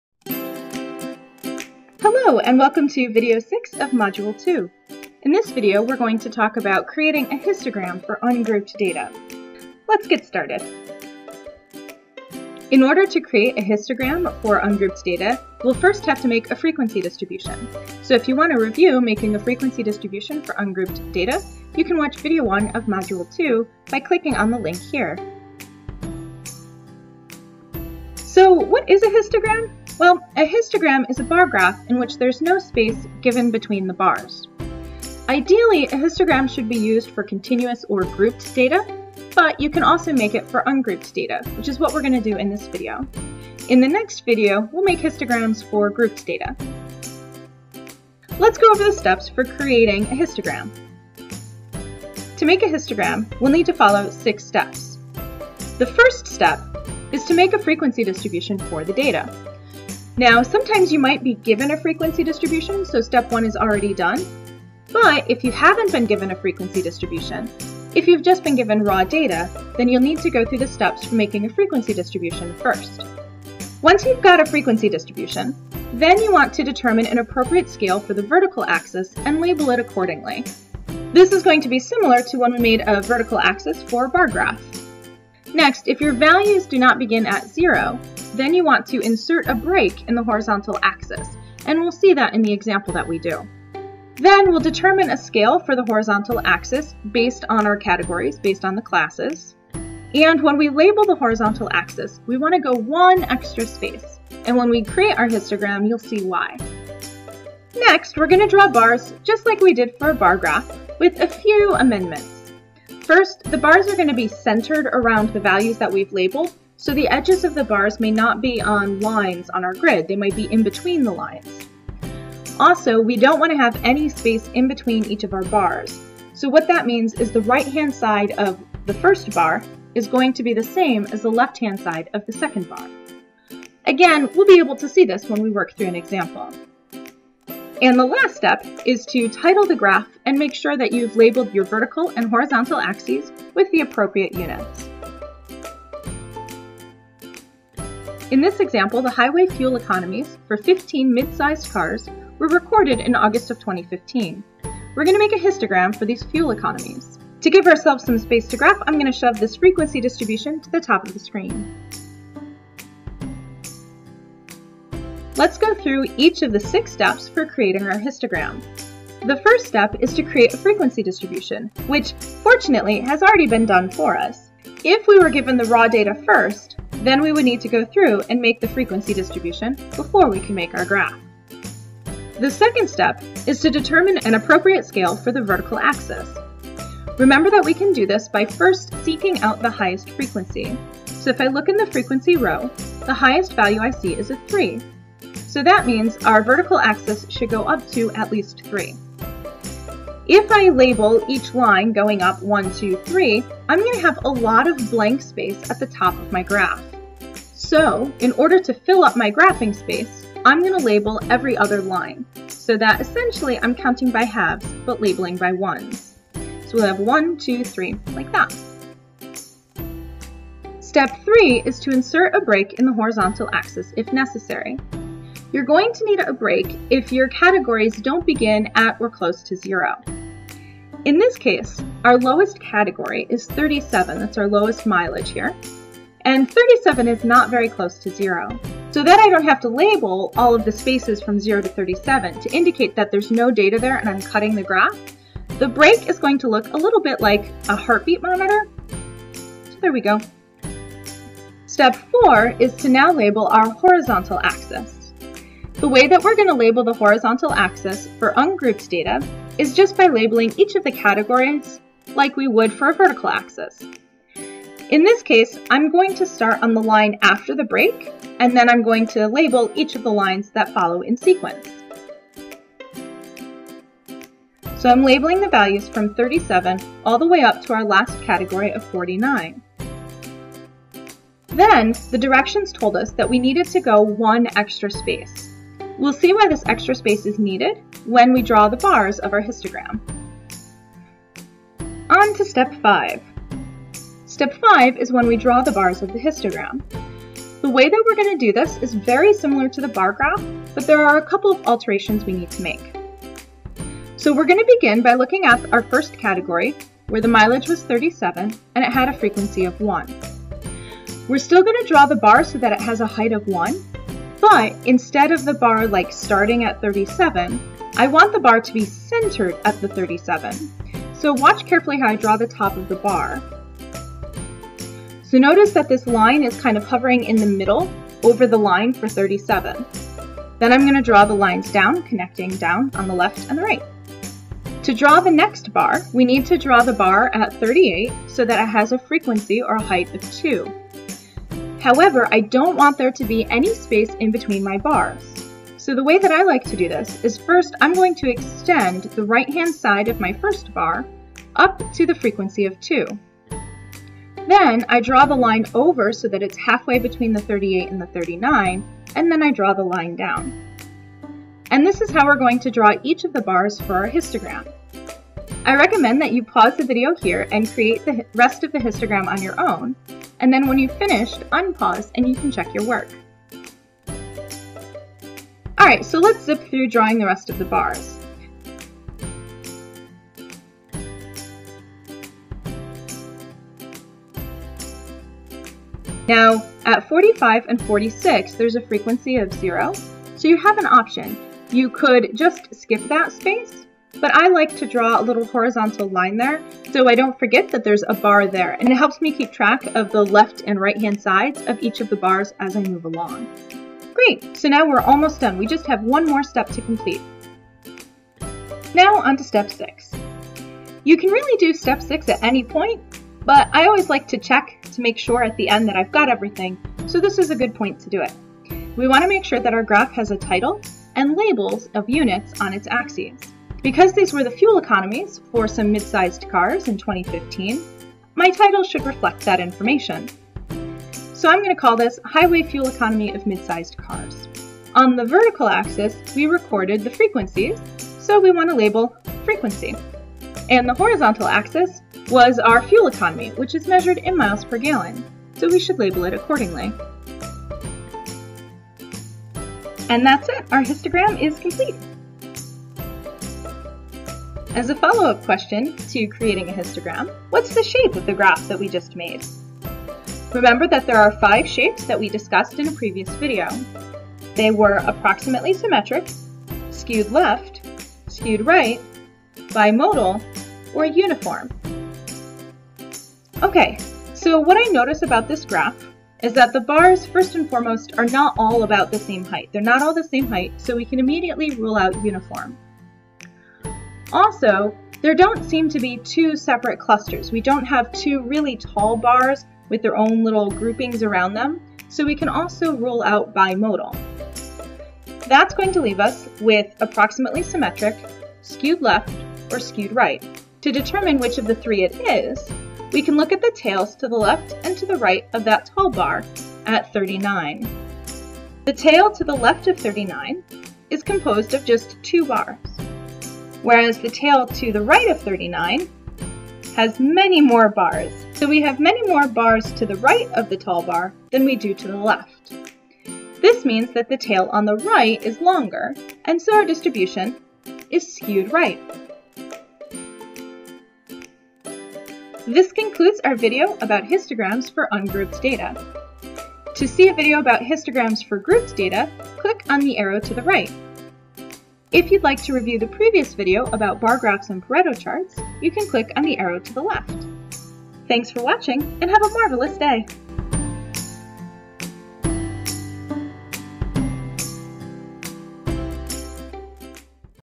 MA110 || Histograms (Ungrouped Data) || Video 2-6
This video explains how to create a histogram for data that is ungrouped.